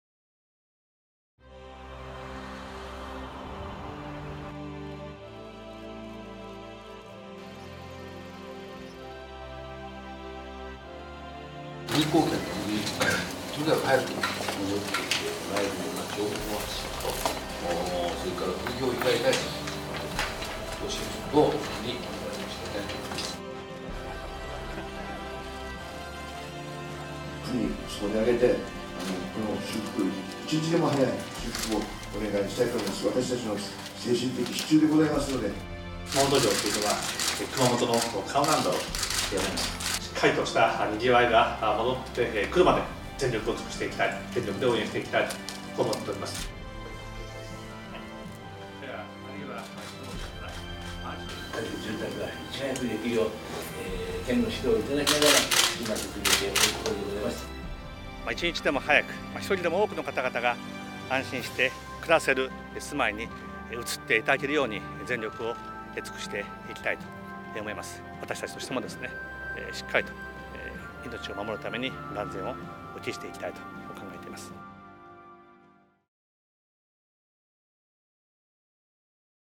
大分県・熊本県訪問 熊本地震による被害状況視察－平成28年4月29日
安倍総理は、熊本地震による被害状況を視察するため、大分県及び熊本県を訪問しました。
総理の一日「熊本地震による被害状況視察のための大分県及び熊本県下訪問」はこちら⇒